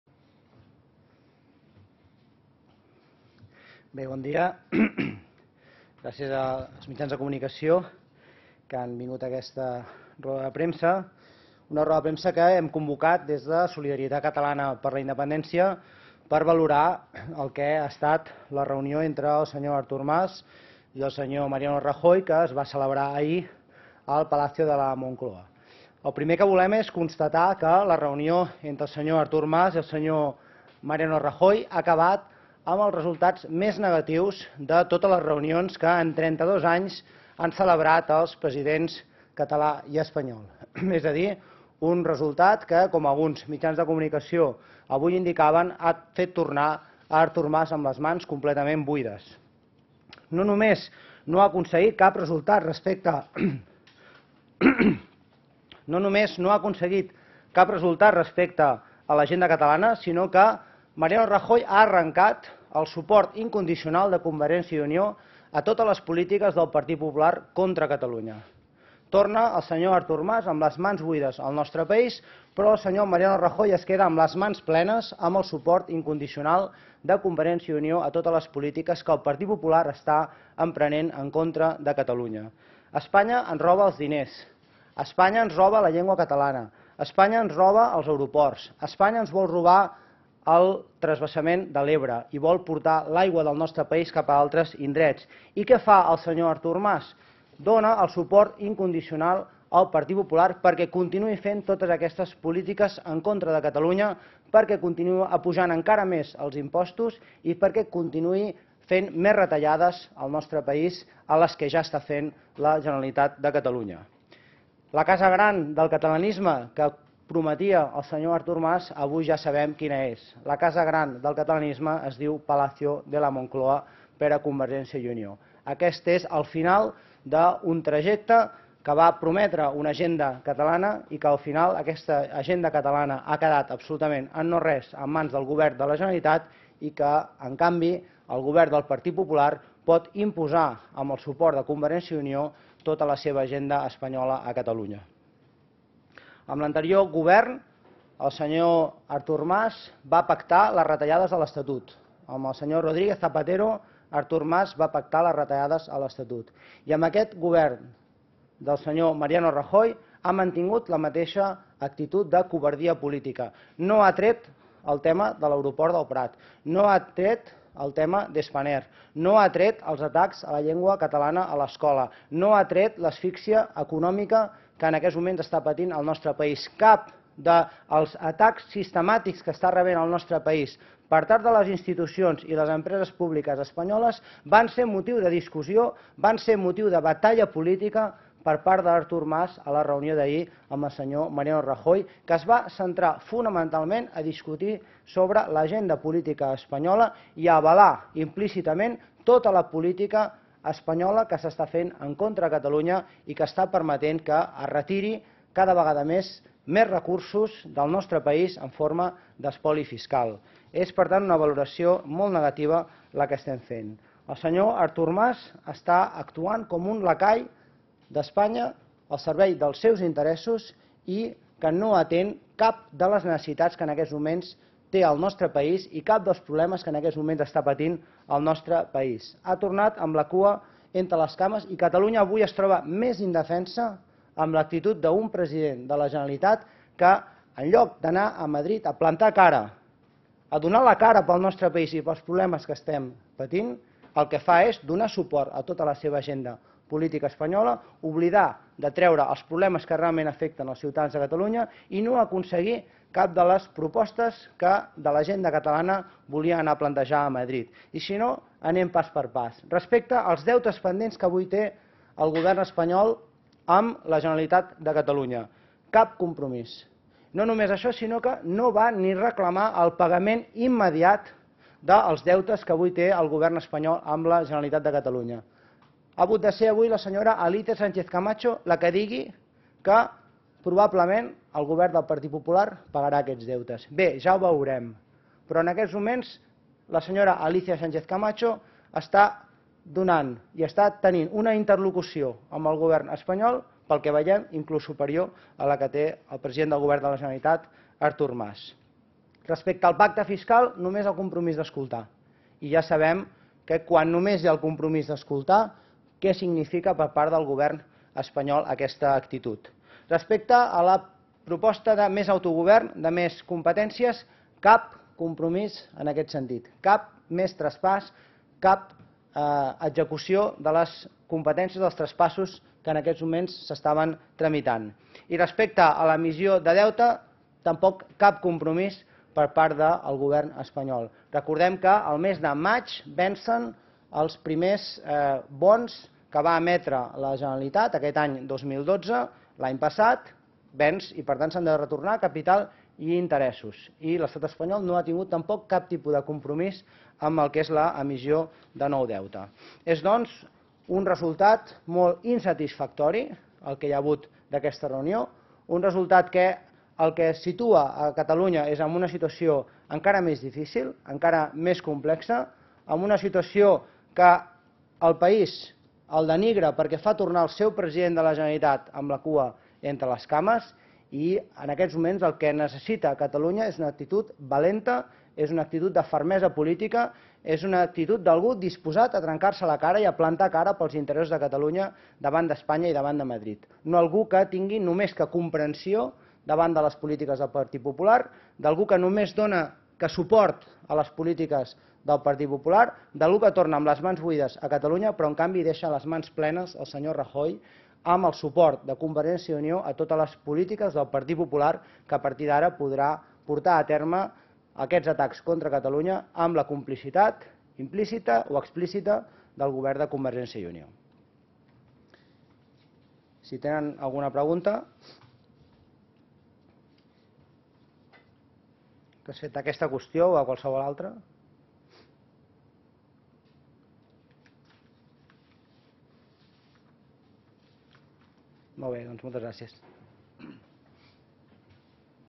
Artur Mas, lacai al servei d'Espanya
El diputat de SI Uriel Bertran ha valorat com a "molt insatisfactori" el resultat de la reunió entre Artur Mas i Mariano Rajoy que va tenir lloc aquest dimecres. Segons ha declarat en una roda de premsa al Parlament, el president català ha tornat amb les mans buides i ha emplenat les de Rajoy perquè aquest li ha arrancat el suport de CiU a les polítiques que està emprenent el govern espanyol contra Catalunya. Per tot això, ha dit Bertran, Mas ha actuat com un lacai al servei de l'estat espanyol. La trobada, ha afegit, ha deixat clar que la casa gran per a CiU és La Moncloa i que el trajecte cap a la transició nacional del què parlava Mas ha quedat en no res.